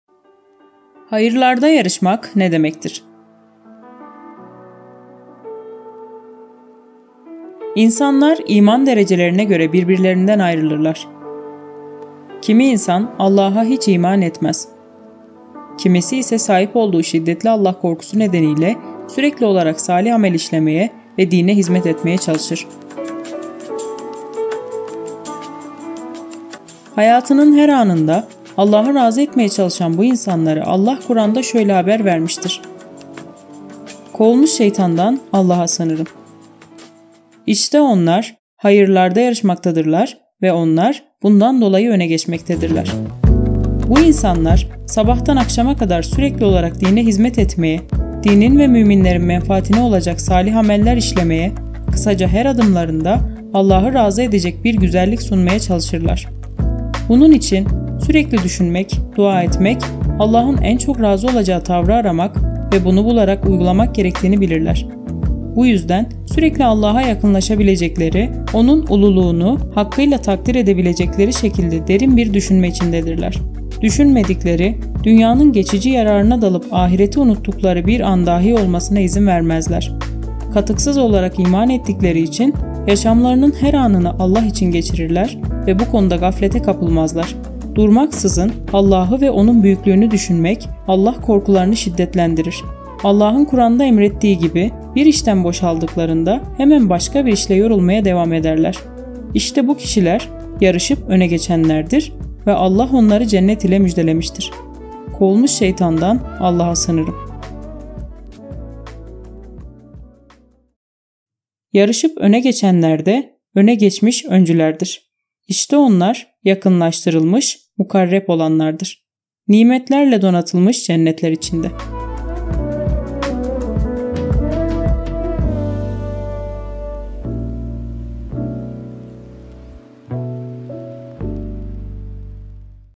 Hayırlarda Yarışmak Ne Demektir?
İnsanlar iman derecelerine göre birbirlerinden ayrılırlar. Kimi insan Allah'a hiç iman etmez. Kimisi ise sahip olduğu şiddetli Allah korkusu nedeniyle sürekli olarak salih amel işlemeye ve dine hizmet etmeye çalışır. Hayatının her anında Allah'ı razı etmeye çalışan bu insanları Allah Kuran'da şöyle haber vermiştir:

"İşte onlar, hayırlarda yarışmaktadırlar ve onlar bundan dolayı öne geçmektedirler." (Müminun Suresi, 61)

Bu insanlar sabahtan akşama kadar sürekli olarak dine hizmet etmeye, dinin ve müminlerin menfaatine olacak salih ameller işlemeye, kısaca her adımlarında Allah'ı razı edecek bir güzellik sunmaya çalışırlar. Bunun için sürekli düşünmek, dua etmek, Allah'ın en çok razı olacağı tavrı aramak ve bunu bularak uygulamak gerektiğini bilirler. Bu yüzden sürekli Allah'a yakınlaşabilecekleri, O'nun ululuğunu hakkıyla takdir edebilecekleri şekilde derin bir düşünme içindedirler. Düşünmedikleri, dünyanın geçici yararına dalıp ahireti unuttukları bir an dahi olmasına izin vermezler. Katıksız olarak iman ettikleri için, yaşamlarının her anını Allah için geçirirler ve bu konuda gaflete kapılmazlar. Durmaksızın Allah'ı ve onun büyüklüğünü düşünmek Allah korkularını şiddetlendirir. Allah'ın Kuran'da emrettiği gibi, bir işten boşaldıklarında, hemen başka bir işle yorulmaya devam ederler. İşte bu kişiler "yarışıp öne geçenler"dir ve Allah onları cennet ile müjdelemiştir:

"Yarışıp öne geçenler de, öne geçmiş öncülerdir. İşte onlar, yakınlaştırılmış (mukarreb) olanlardır. Nimetlerle-donatılmış cennetler içinde;" (Vakıa Suresi, 10-12)